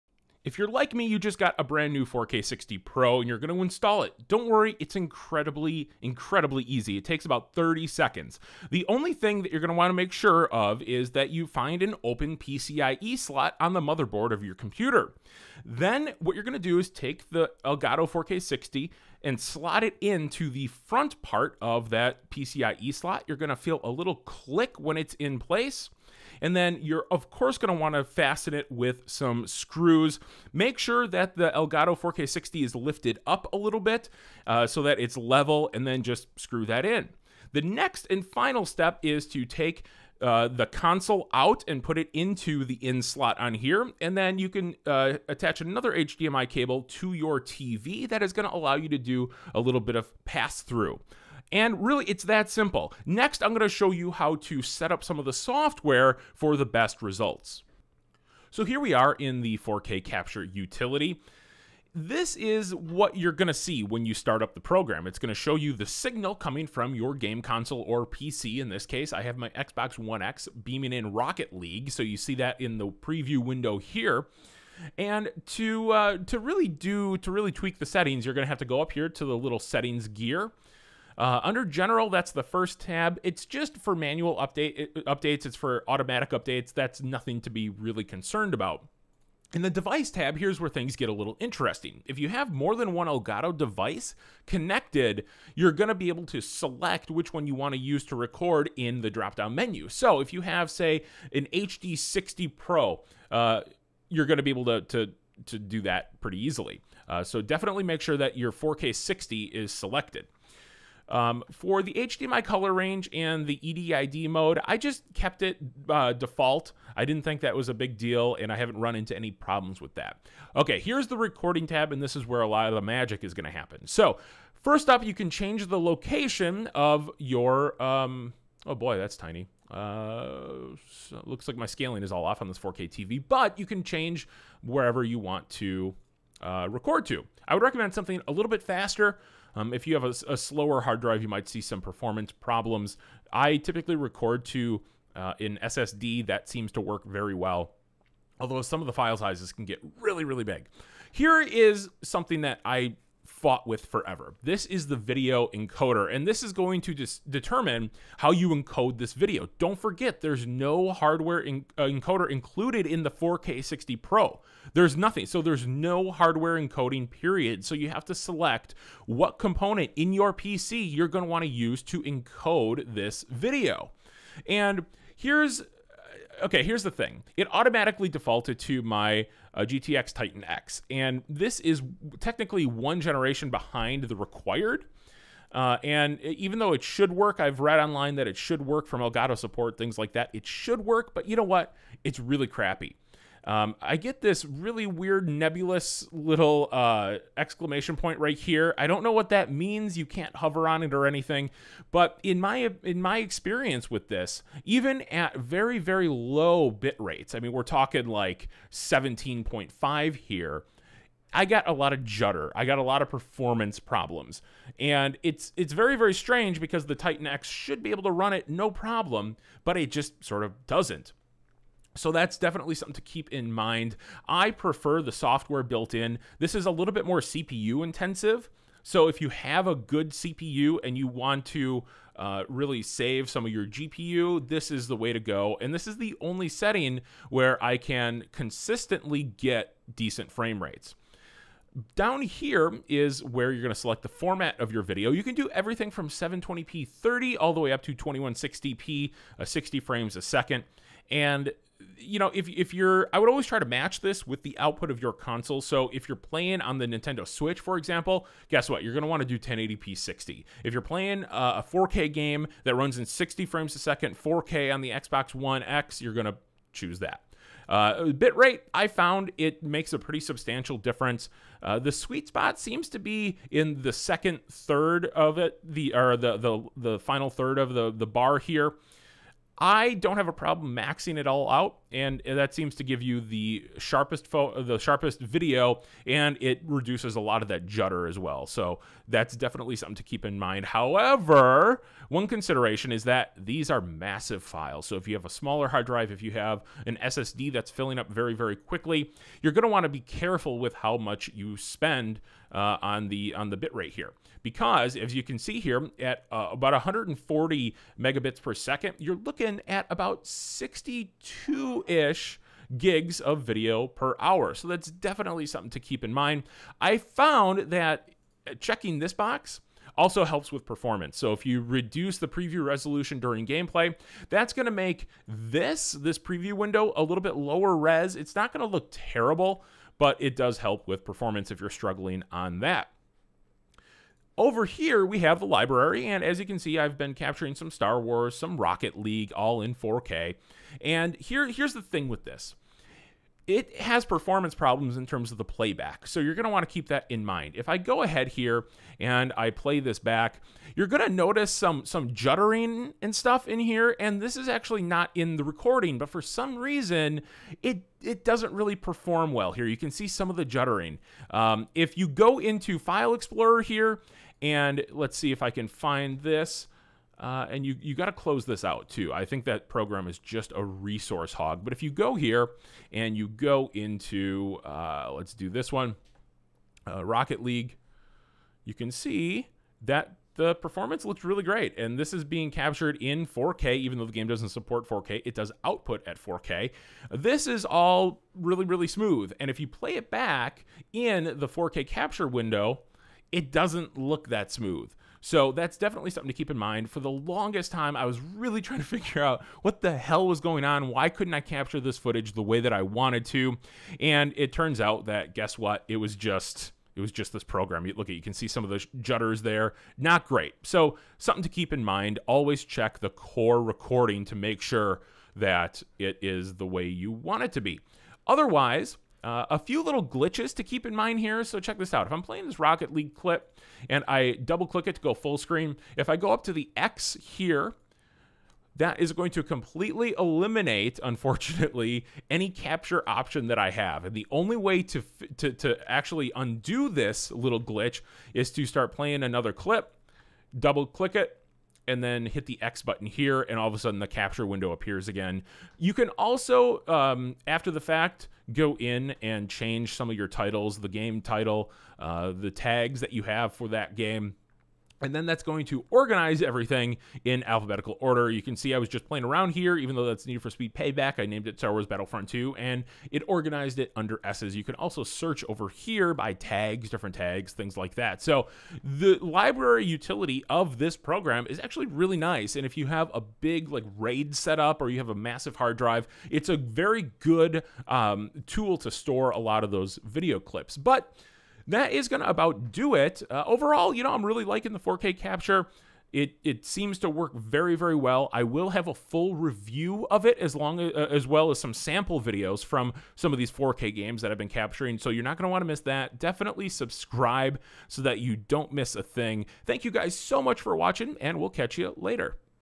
How to install Elgato 4K60 Pro, best settings for 4K Capture Utility
The Elgato 4K60 Pro is here, and we walk you through how to install it. We also take a look at the best settings for Elgato's 4K Capture Utility. Eliminate stuttering, judder, and performance problems with these settings.